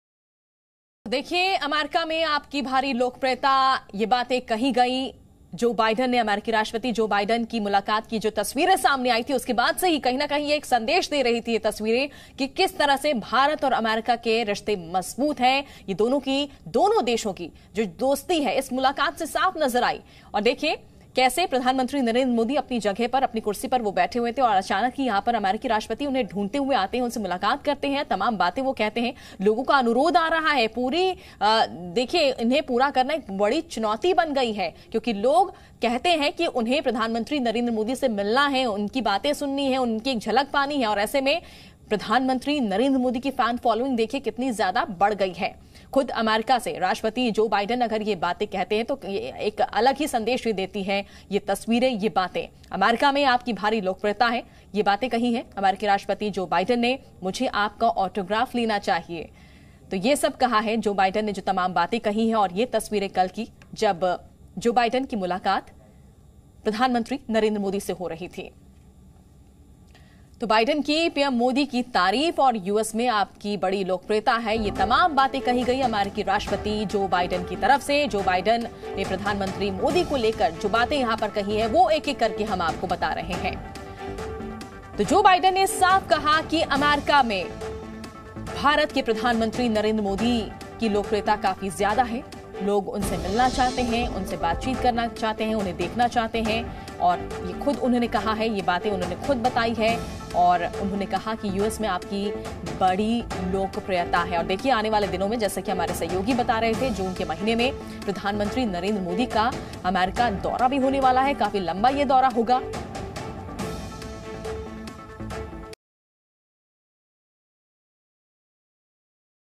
G7 Summit 2023: Quad में दिखी PM Modi की लोकप्रियता, America President Joe Biden ने की तारीफ
G7 Summit 2023:  दुनिया के सबसे ताकतवर देश अमेरिका के राष्ट्रपति जो बाइडन तो प्रधानमंत्री मोदी के इतने प्रशंसक बन चुके हैं कि वह उनका ऑटोग्राफ लेने की बात करने लगे हैं।

About Channel